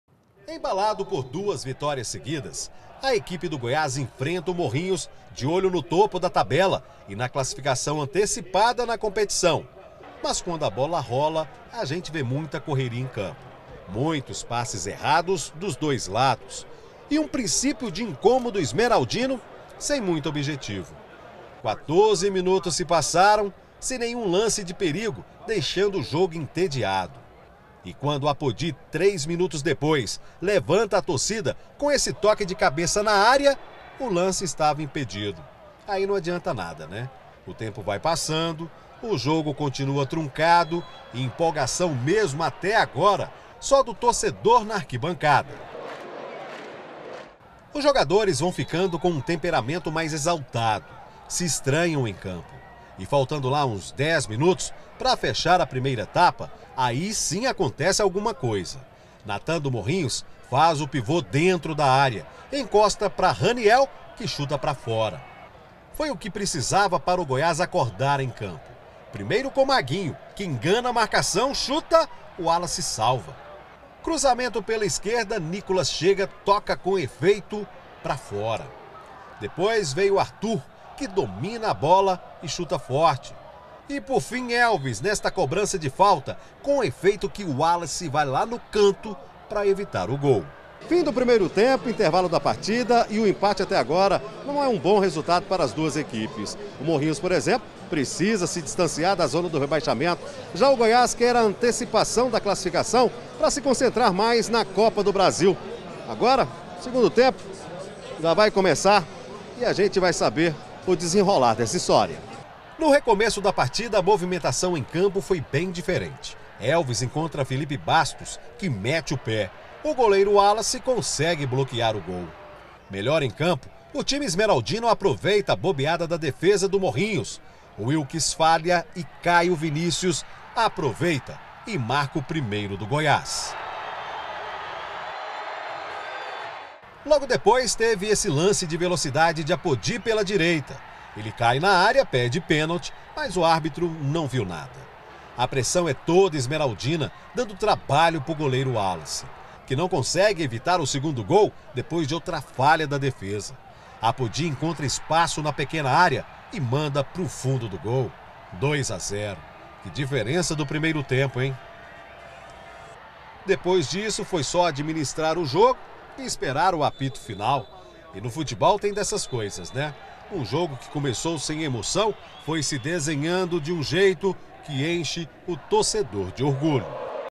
CLASSIFICADO: GOIÁS BATE O MORRINHOS E VOLTA A LIDERANÇA
GOIÁS BATE O MORRINHOS VOLTA A LIDERANÇA E SE CLASSIFICA PARA AS QUARTAS DE FINAL.